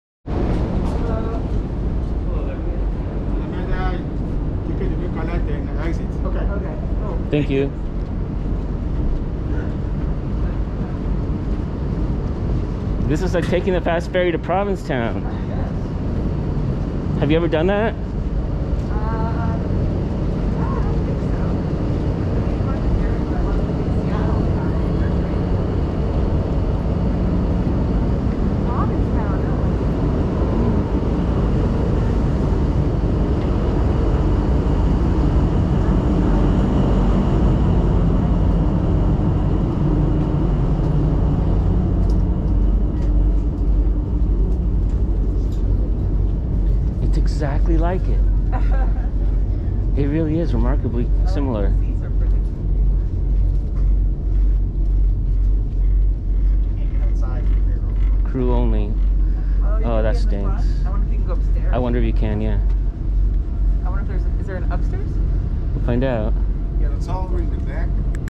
Getting on the San Francisco ferry to Oakland/Alameda
Taking the ferry from San Francisco to Alameda, California, on Monday, April 18, 2022.